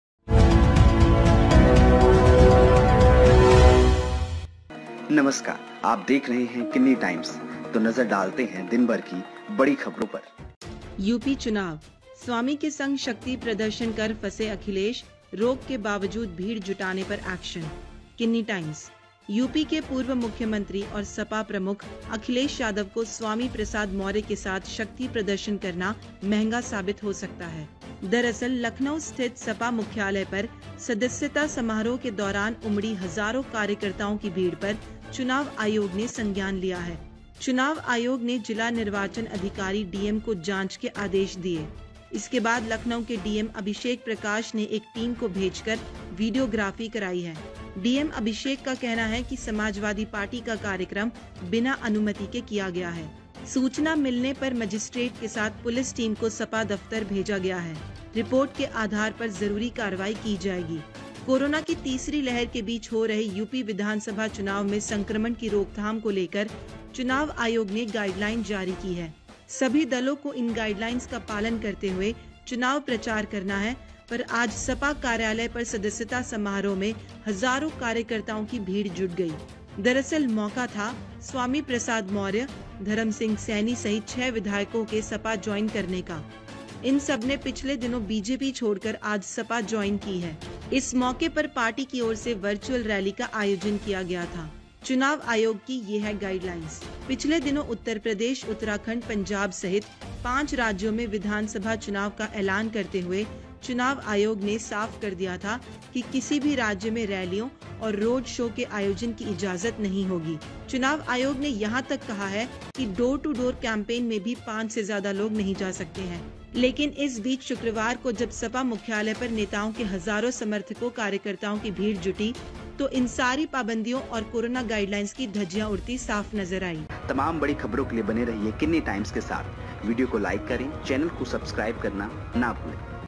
Akhilesh Yadav ने तोड़े सारे Covid Protocol, चुनाव आयोग ले सकता है Action
UP Election ,स्वामी के संग शक्ति प्रदर्शन कर फंसे Akhilesh Yadav, रोक के बावजूद भीड़ जुटाने पर Action
Kinni Times यूपी के Ex CM और SP प्रमुख Akhilesh Yadav को Swami Prasad Maurya के साथ शक्ति प्रदर्शन करना महंगा साबित हो सकता है। दरअसल Lucknow स्थित सपा मुख्‍यालय पर सदस्यता समारोह के दौरान उमड़ी हजारों कार्यकर्ताओं की भीड़ पर चुनाव आयोग ने संज्ञान लिया है। चुनाव आयोग ने जिला निर्वाचन अधिकारी डीएम को जांच के आदेश दिए। इसके बाद लखनऊ के डीएम अभिषेक प्रकाश ने एक टीम को भेजकर वीडियोग्राफी कराई है। डीएम अभिषेक का कहना है कि समाजवादी पार्टी का कार्यक्रम बिना अनुमति के किया गया है। सूचना मिलने पर मजिस्ट्रेट के साथ पुलिस टीम को सपा दफ़्तर भेजा गया है। रिपोर्ट के आधार पर ज़रूरी कार्रवाई की जाएगी।
कोरोना की तीसरी लहर के बीच हो रहे यूपी विधानसभा चुनाव में संक्रमण की रोकथाम को लेकर Election Commison आयोग ने गाइडलाइन जारी की है। सभी दलों को इन गाइडलाइंस का पालन करते हुए चुनाव प्रचार करना है पर आज सपा कार्यालय पर सदस्यता समारोह में हजारों कार्यकर्ताओं की भीड़ जुट गई। दरअसल मौका था, स्‍वामी प्रसाद मौर्य, धर्म सिंह सैनी सहित छह विधायकों के सपा ज्‍वाइन करने का। इन सबने पिछले दिनों बीजेपी छोड़कर आज सपा ज्‍वाइन की है। इस मौके पर पार्टी की ओर से Virtual Rally का आयोजन किया गया था।
चुनाव आयोग की ये है गाइड लाइंस पिछले दिनों उत्तर प्रदेश, उत्तराखंड, पंजाब सहित पांच राज्यों में विधानसभा चुनाव का ऐलान करते हुए चुनाव आयोग ने साफ कर दिया था कि किसी भी राज्य में रैलियों और रोड शो के आयोजन की इजाजत नहीं होगी। इसके अलावा किसी नुक्कड़ सभा का आयोजन भी सार्वजनिक स्थानों पर नहीं किया जा सकेगा। साइकिल रैली और बाइक रैली और पदयात्रा जैसी चीजों पर भी रोक रहेगी। बाइक रैली पर भी पाबंदी रहेगी। रात 8 बजे के बाद चुनाव प्रचार पर रोक रहेगी। चुनाव आयोग ने चुनावी पार्टियों से कहा है कि वो ज्यादा से ज्यादा वर्चुअल रैली या डिजीटल रैली पर जोर दें। ये सभी पाबंदियां 15 जनवरी तक के लिए लागू की गई थीं। Covid Guidelines
चुनाव आयोग ने साफ किया था कि 15 जनवरी के बाद इसकी समीक्षा की जाएगी और फिर आगे इसपर फैसला लिया जाएगा। चुनाव आयोग ने यहां तक कहा है कि डोर टू डोर कैंपेन में भी 5 से ज्यादा लोग नहीं जा सकते हैं। लेकिन इस बीच शुक्रवार को जब सपा मुख्‍यालय पर नेताओं के हजारों समर्थकों-कार्यकर्ताओं की भीड़ जुटी तो इन सारी पाबंदियों और कोरोना गाइडलाइन की धज्जियां उड़ती साफ नज़र आईं।